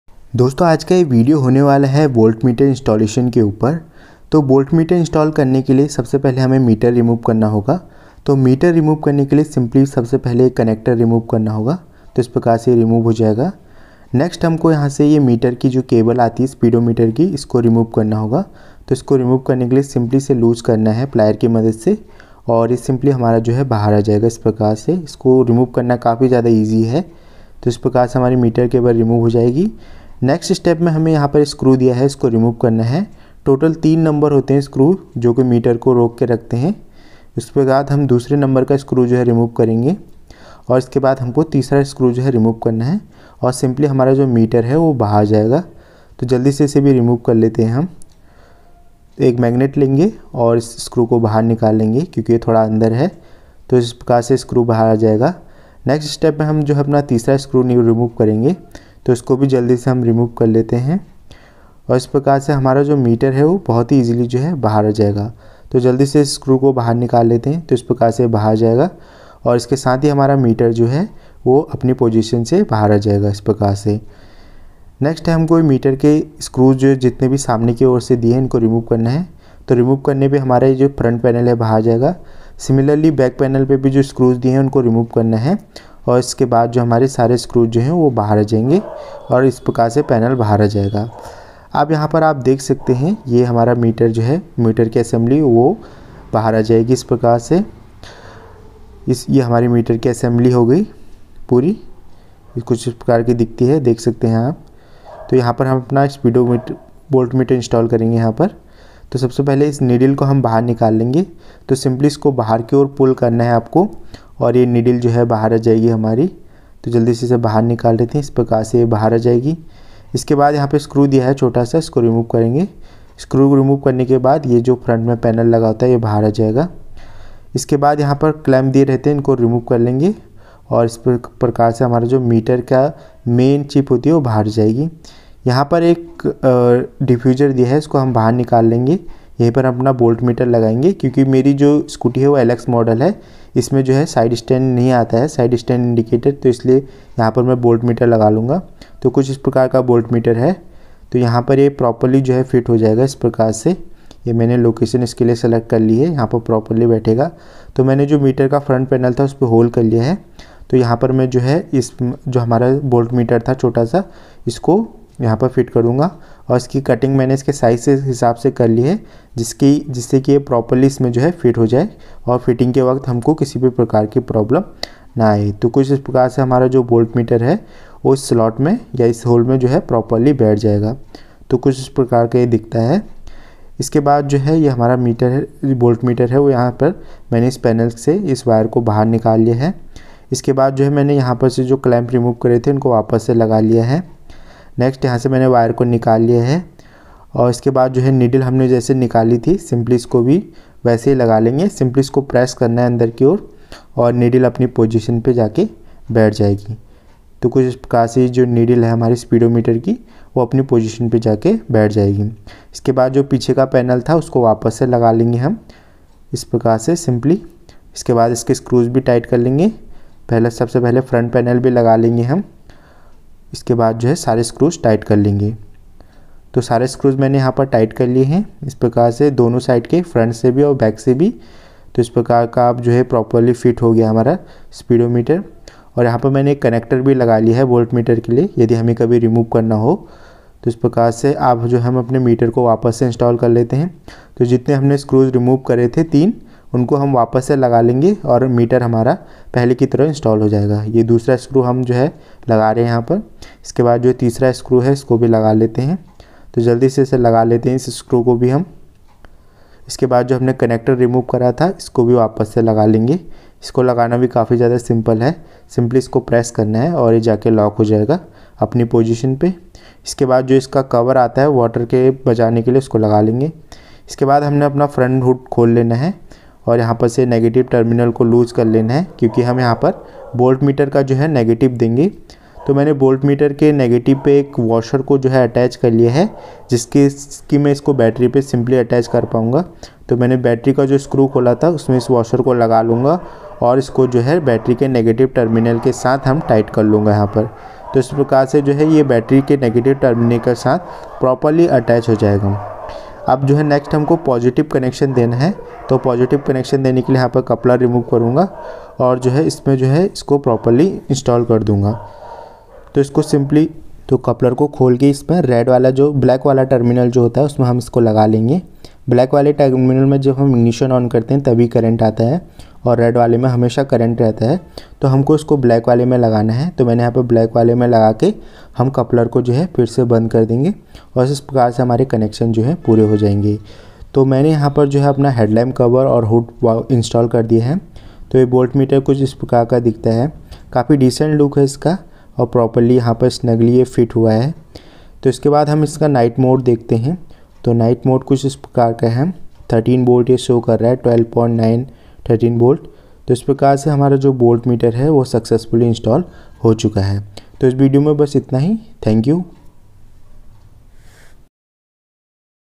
Voltmeter installation inside speedometer console in maestro edge/activa/jupiter||Hindi||Waterproof
Hello guys, today's video is going to be about voltmeter installation.

Thankyou.